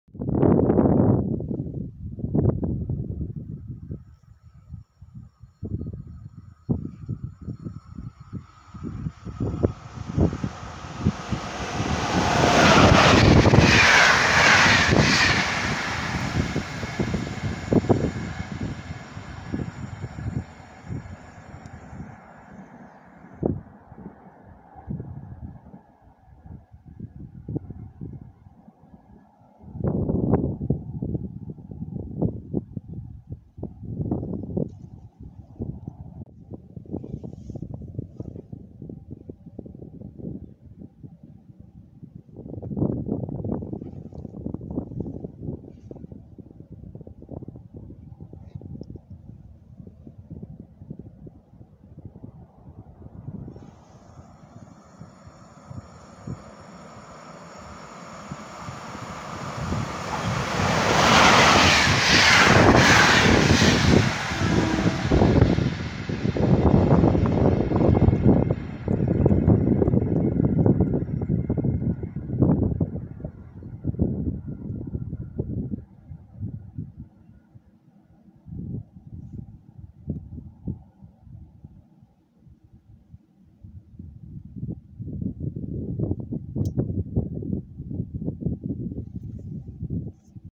Dwa "Bolki" w Raszówce.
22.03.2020 Przeloty składu IC Bolko w obydwu relacjach przez stację Raszówka.
1. EP07-339 z IC Bolko Lublin Gł - Szczecin Gł przelatuje w kierunku Lubina.
2. EP07-1032 z IC Bolko Szczecin Gł - Lublin Gł przelatuje w kierunku Legnicy. Obok stoi przepuszczający go Impuls KD do Głogowa.